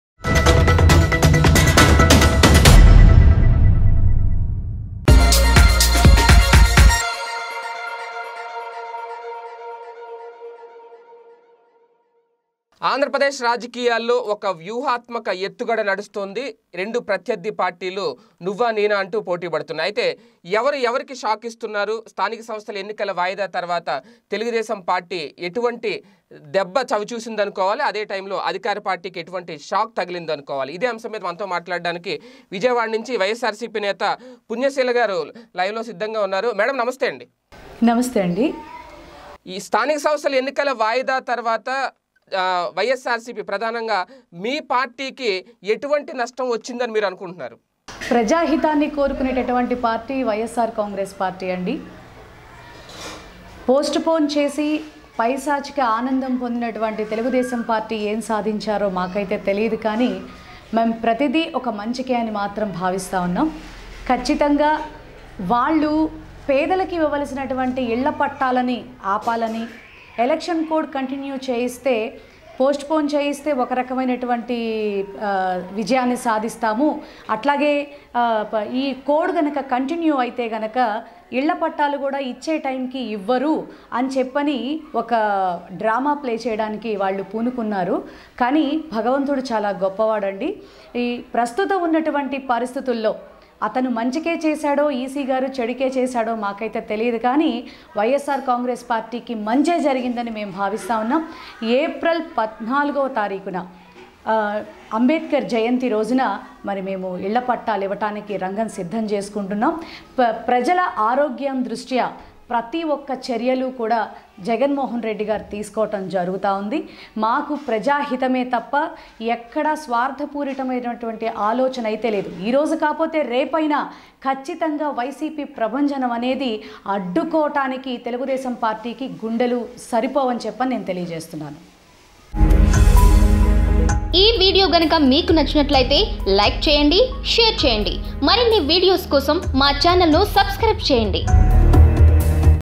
YSRCP Leader Comment On Postpone Of Local Body Elections | ఇళ్ల పట్టాల పంపిణీ చేసి తీరుతాం
ఇళ్ల పట్టాల పంపిణీ చేసి తీరుతాం.. ఏం పీక్కుంటారో పీక్కోండి | YSRCP Leader Comment On Postpone Of Local Body Elections